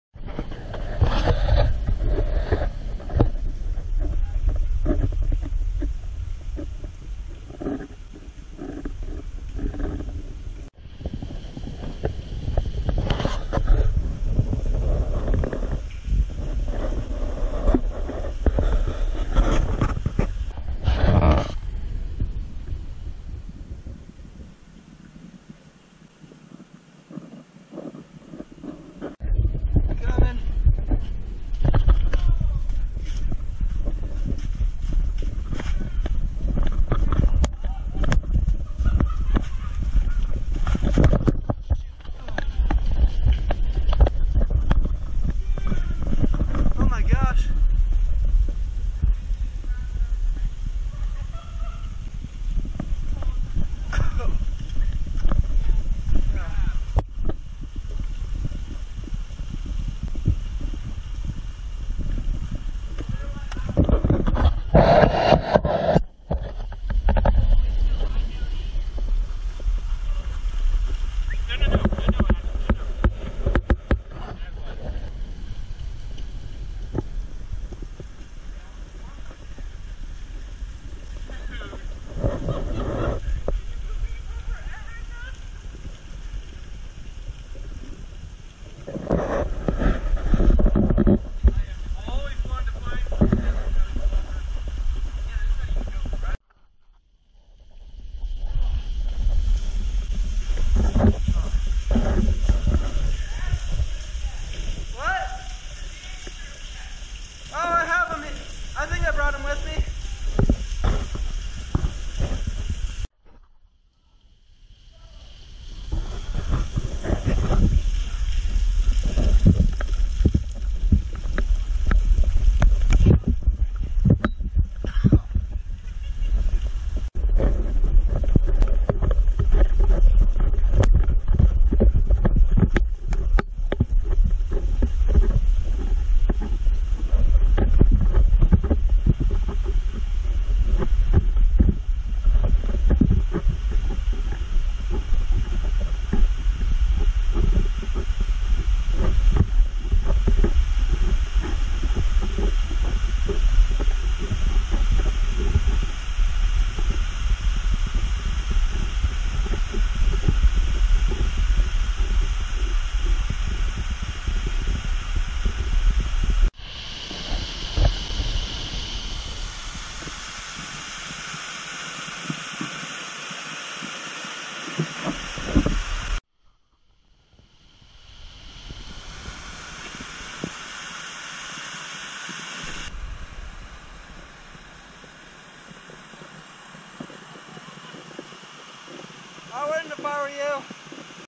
Exploring the Makushin Glacier Caves (4K)
Here is a compilation of some 4K videos I took of the glacier on Makushin Volcano in Unalaska, AK, and what we found while exploring its caves. To see the pH of the boiling acid water we found, check out my Instagram page @batdan2018.